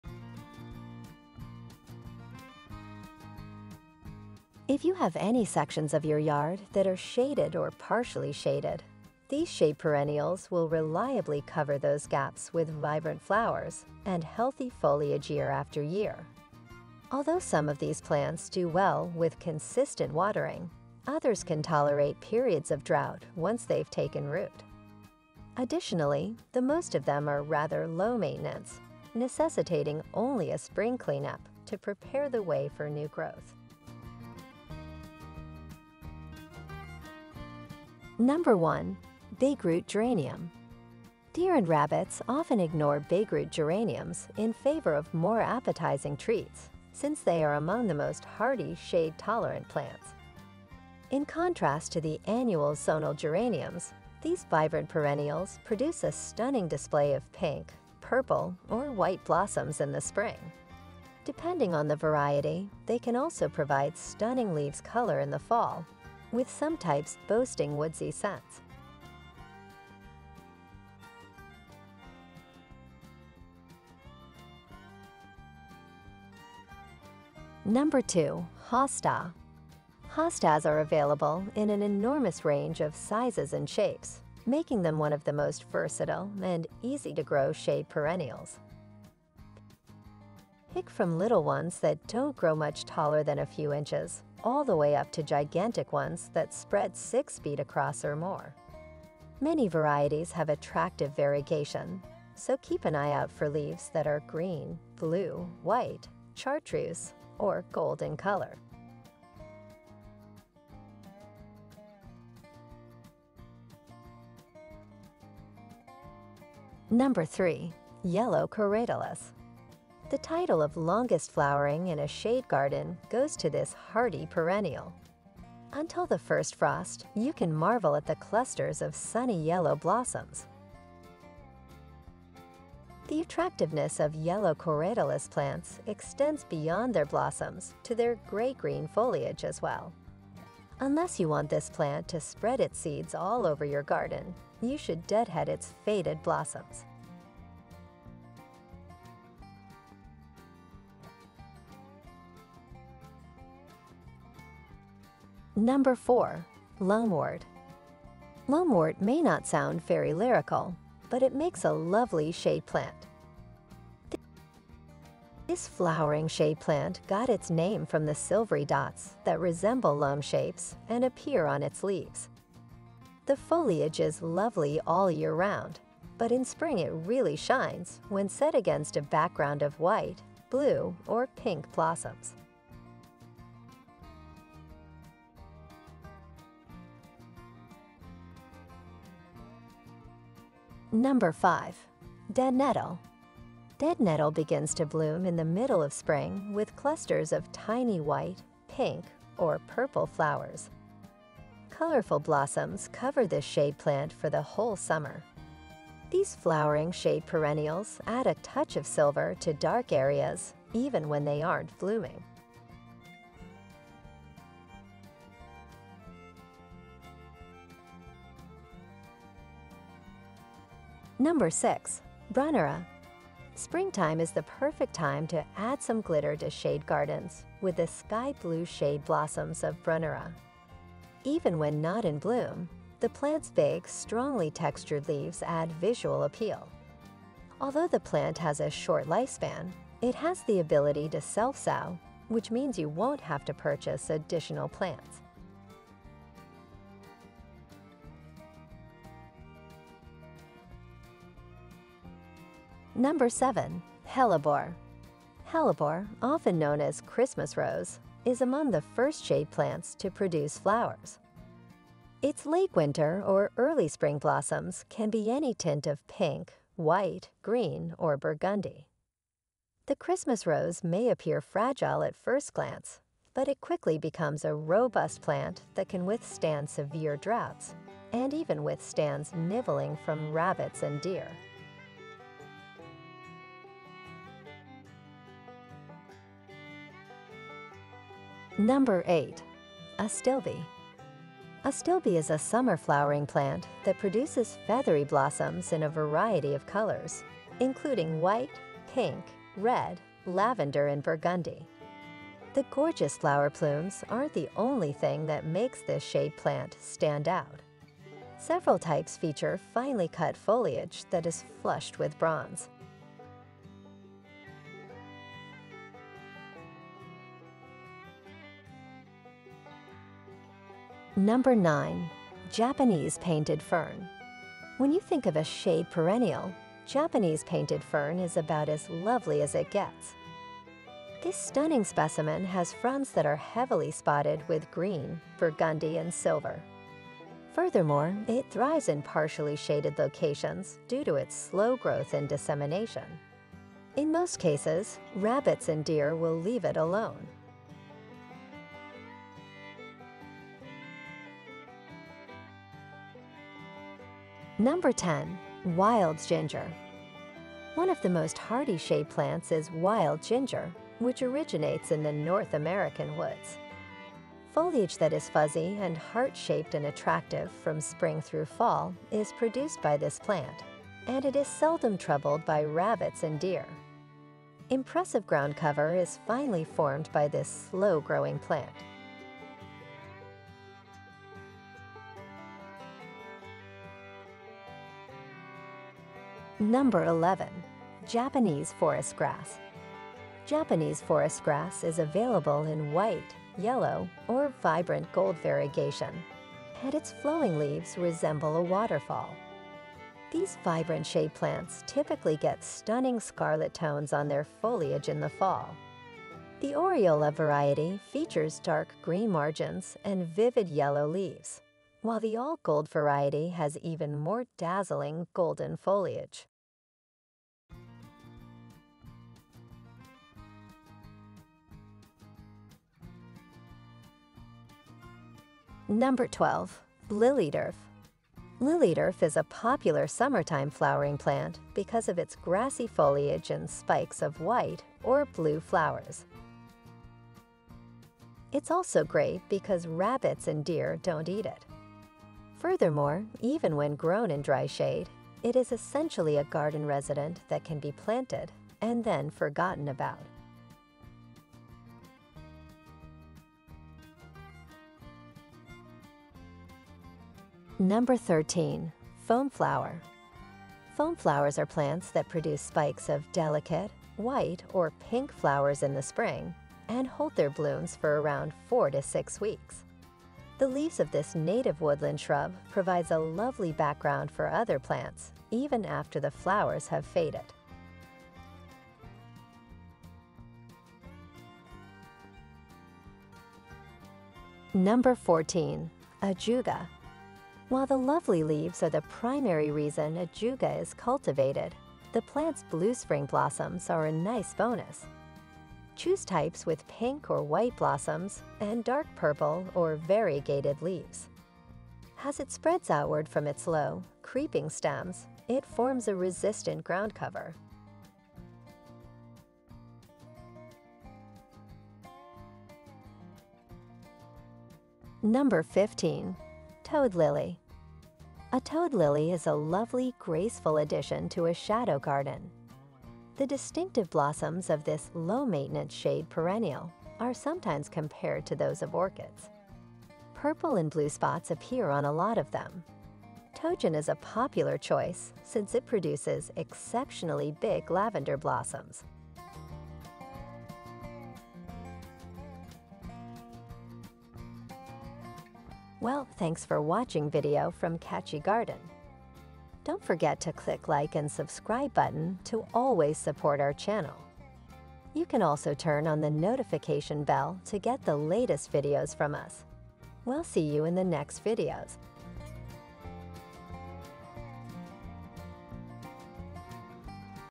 EASY TO GROW! TOP 15 PERENNIALS THAT THRIVE IN SHADE! 🌸🌳🌼 // Gardening Ideas
If you have any sections of your yard that are shaded or partially shaded, these shade perennials will reliably cover those gaps with vibrant flowers and healthy foliage year after year.

Although some of these plants do well with consistent watering, others can tolerate periods of drought once they've taken root.

Additionally, the most of them are rather low-maintenance, necessitating only a spring cleanup to prepare the way for new growth.
⏱  TOP 15 PERENNIALS THAT THRIVE IN SHADE!
00:00 Intro
00:36 Bigroot Geranium
01:15 Hosta
01:55 Yellow Corydalis
02:33 Lungwort
03:09 Deadnettle
03:39 Brunnera
04:11 Hellebore
04:47 Astilbe
05:19 Japanese Painted Fern
05:53 Wild Ginger
06:25 Japanese Forestgrass
07:04 Lilyturf
07:36 Foamflower
08:02 Ajuga
08:33 Toad Lily
09:05 Outro

garden crossings, gardeners world, garden media, horticulture, oklahoma gardening